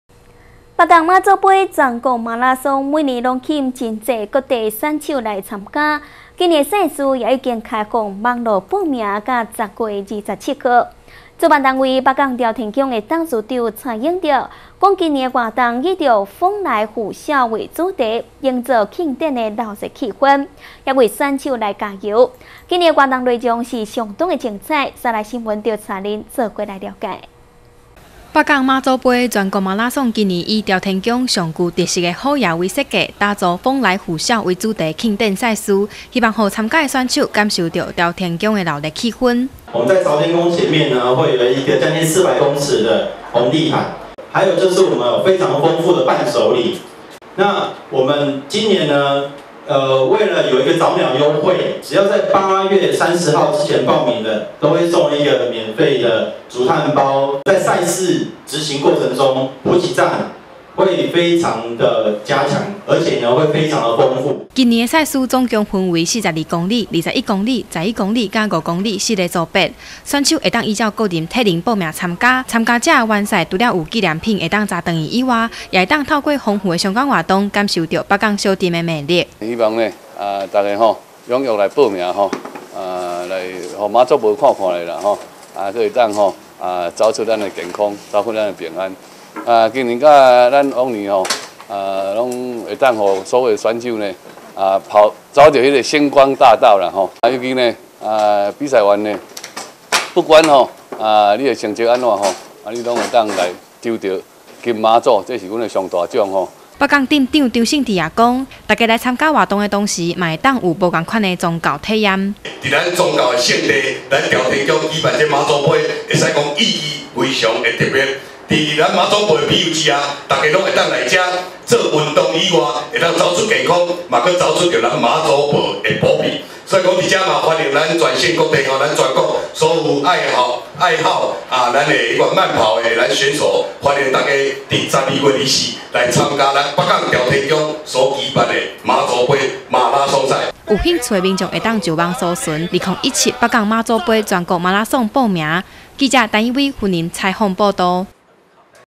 雲林新聞網-北港媽祖盃馬拉松報名開跑
北港媽祖盃全國馬拉松每年吸引很多各地選手參加，今年賽事報名也已經開放網路報名到10月27號。主辦單位北港朝天宮董事長蔡咏锝說，今年活動以「風來虎嘯」為主題，營造慶典的熱鬧氛圍為選手加油。活動內容不只有大家期待的「星光大道」賽道，還特別與在地店家結合推出消費優惠活動，另外也在賽事中增設「特殊裝扮獎」，希望讓參加的選手有一個豐富又趣味的馬拉松印象。【以上新聞可從 ｔｏｐＮＥＷＳ ＡＰＰ收視】